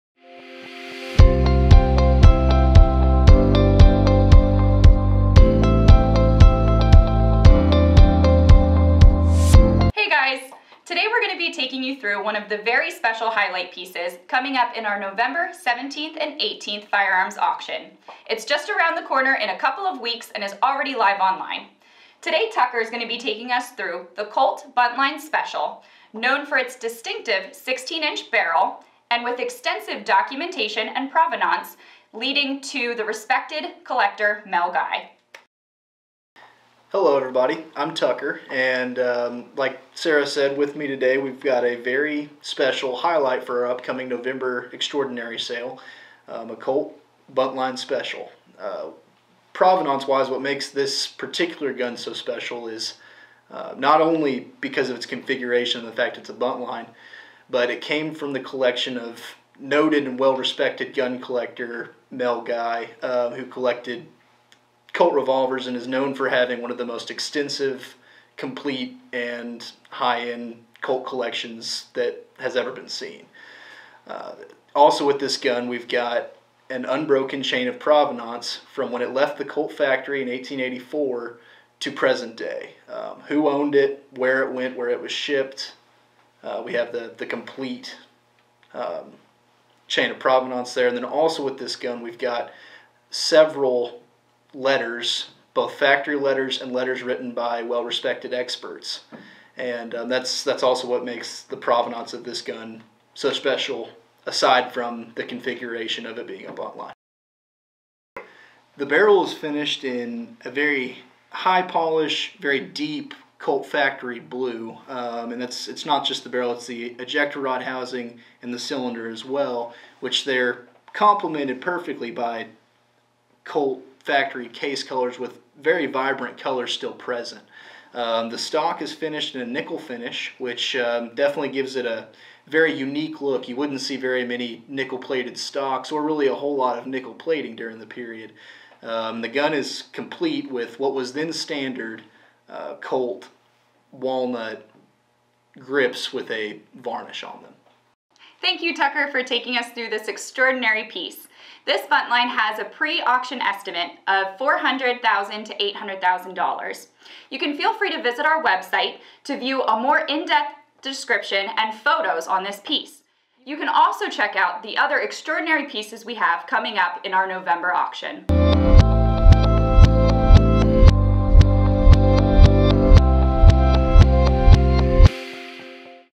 Noted Collector Mel Guy's Legendary Colt "Buntline Special" Single Action Army Revolver
Two days until noted collector, Mel Guy’s legendary Colt “Buntline Special” headlines our Extraordinary Firearms two day sale. A collector of more than 300 antique Colt six-shooters, Mel spent decades trying to track down an elusive Buntline. Story has it that 19th-century dime novelist Edward Judson once commissioned Colt to produce five single-action Army revolvers – known as Buntline Specials – to present to Dodge City lawmen in appreciation for sharing their frontier yarns with him.

Hear from Morphy’s Firearm’s Compliance Specialist, Tucker Nelson on the specifications on the Buntline as well as its rarity. This Buntline is one of the most renowned and coveted of all known Colt single-action Army Revolvers and is estimated at $400,000 - $800,000